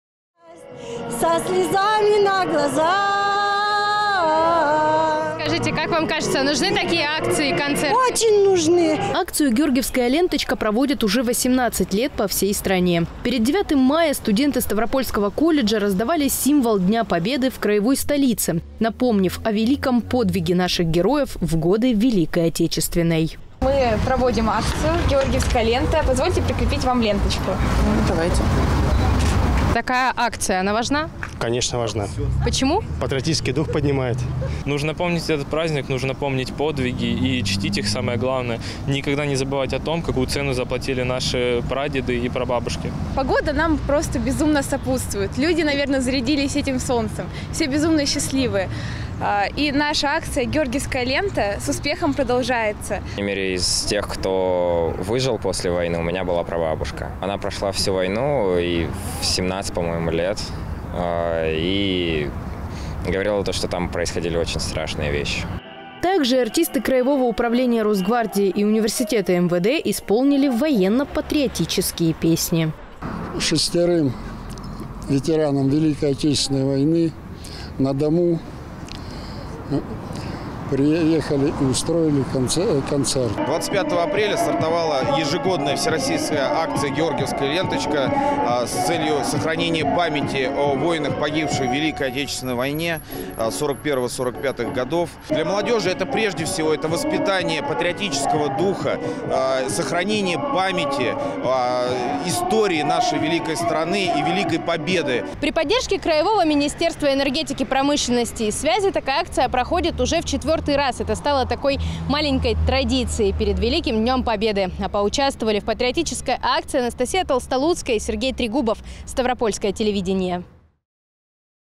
В Ставрополе провели всероссийскую акцию «Георгиевская ленточка»
Студенты ставропольского колледжа вручили горожанам символы победы.

В Ставрополе провели всероссийскую акцию «Георгиевская ленточка», которая стартовала 25 апреля. Студенты ставропольского колледжа перед 9 мая раздавали символ Дня Победы в краевой столице.

Акцию «Георгиевская ленточка» проводят уже 18 лет по всей стране, напоминая о великом подвиге наших героев в годы Великой Отечественной.

Организовывают акцию в Ставрополе уже 4 раз. Проходит она при поддержке краевого Министерства энергетики, промышленности и связи.

«Стартовала ежегодная всероссийская акция «Георгиевская ленточка» 25 апреля. Проводят её с целью сохранить память о героях, погибших в Великой Отечественной войне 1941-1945-х годов. Для молодёжи, прежде всего, это воспитание патриотического духа, сохранение памяти и истории нашей великой страны и великой победы», - рассказал министр энергетики, промышленности и связи Ставропольского края Иван Ковалёв.

Также для жителей краевой столицы организовали концерт. Там артисты краевого управления Росгвардии и университета МВД исполнили военно-патриотические песни.